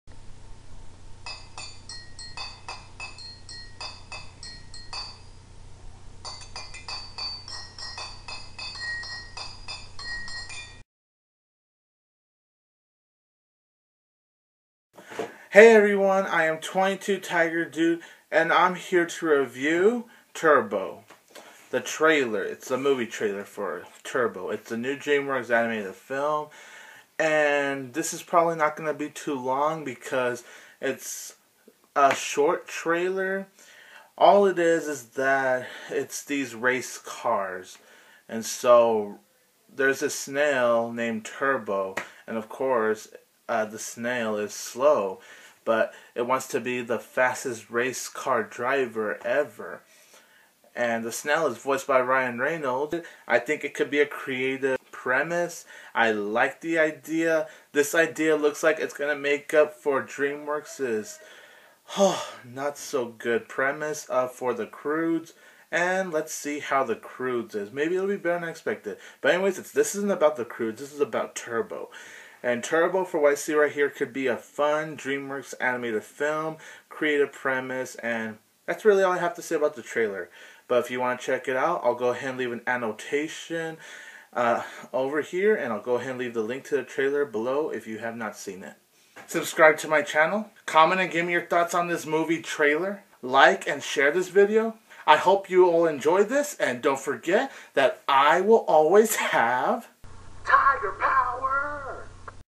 Turbo trailer review
22tigerdude reviews the trailer to the DreamsWorks animated film about a snail who wants to be a racecar driver releasing in theaters on July 19, 2013. What are his thoughts on this trailer? find out here.